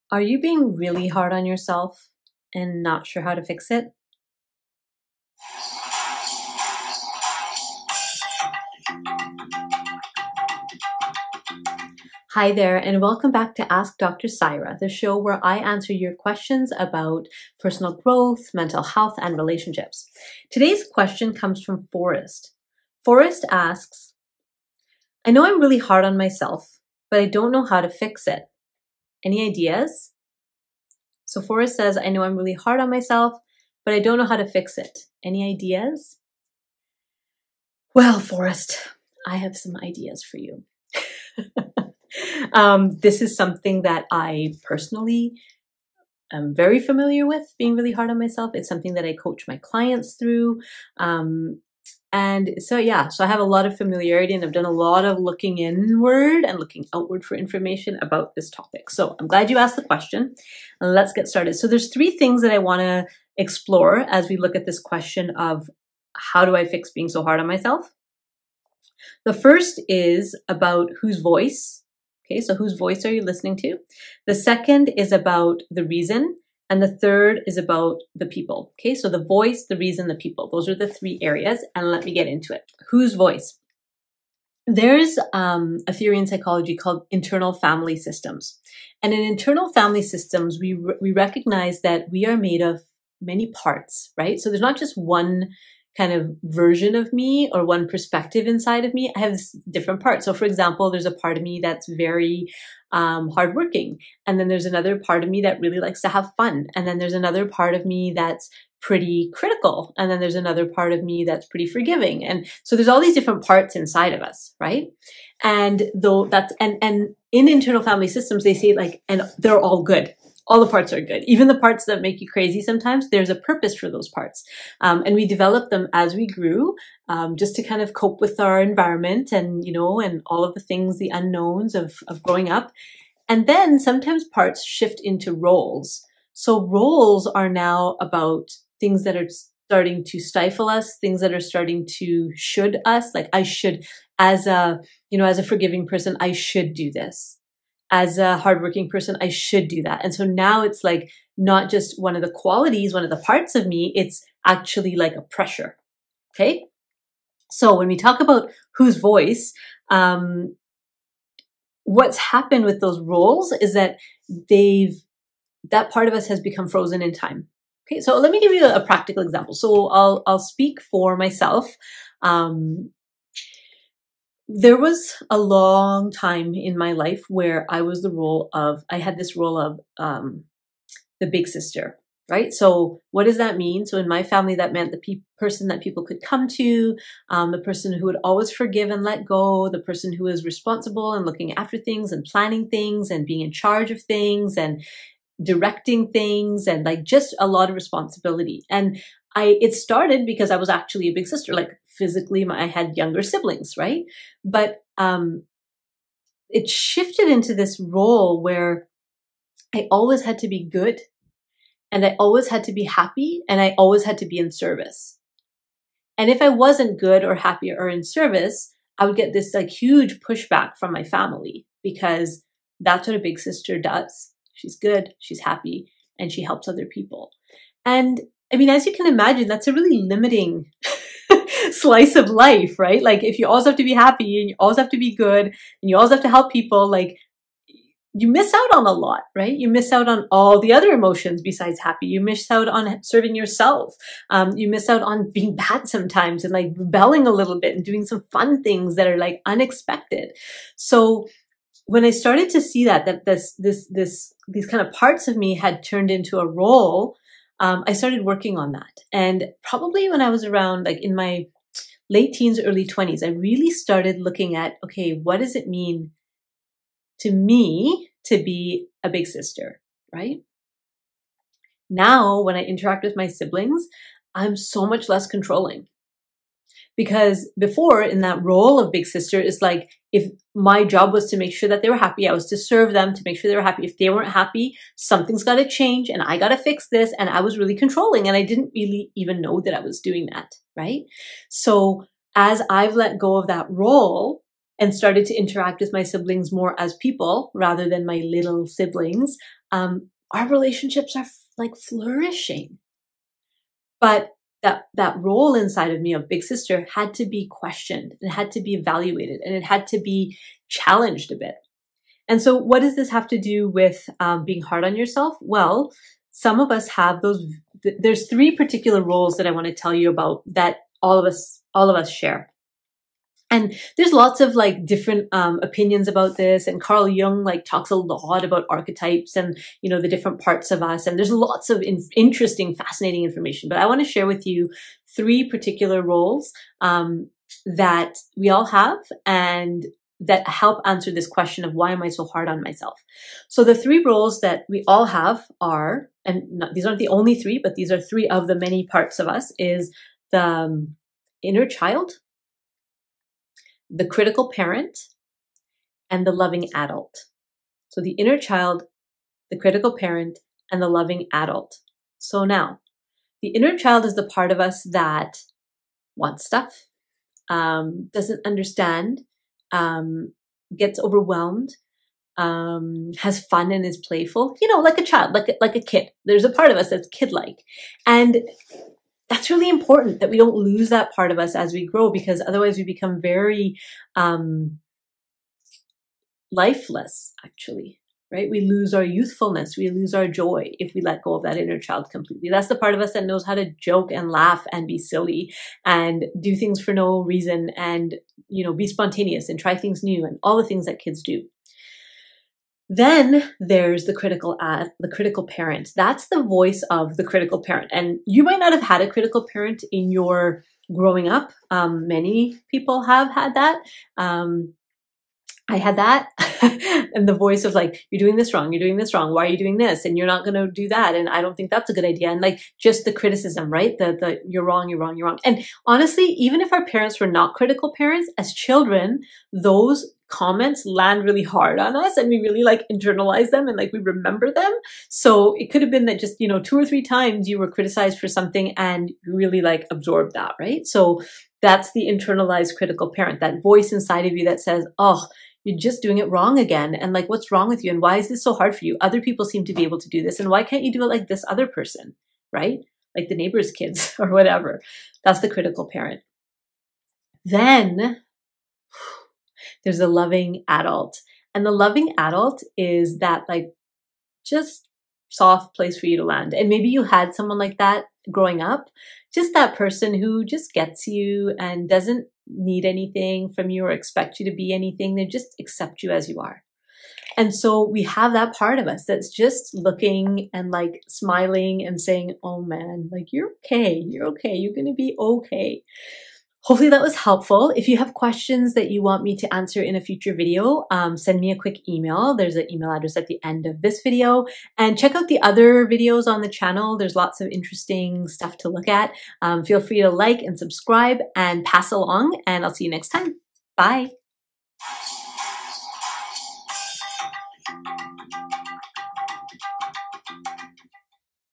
Why are you so hard on yourself? (Part 1 of 2)
This week's question is about tools that can help us stop being so hard on ourselves.

Part 2 of 2: coming soon

In this part 1 video, Dr. Saira talks about the reason we are so hard on ourselves and understand whose voices we are listening to.

00:00 Intro
01:23 Whose voice are you listening to? Parts & Roles
05:16 The Inner-Child, The Critical Parent, and The Loving Adult